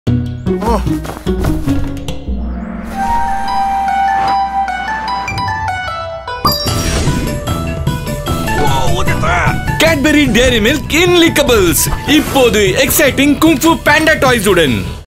Cadbury Dairy Milk - Kung Fu Panda Toys In Lickables | Tamil (15 secs)
Ready to become the next Dragon Warrior? Get an exciting new range of Kung Fu Panda toys with Cadbury Dairy Milk Lickables. Go collect them all.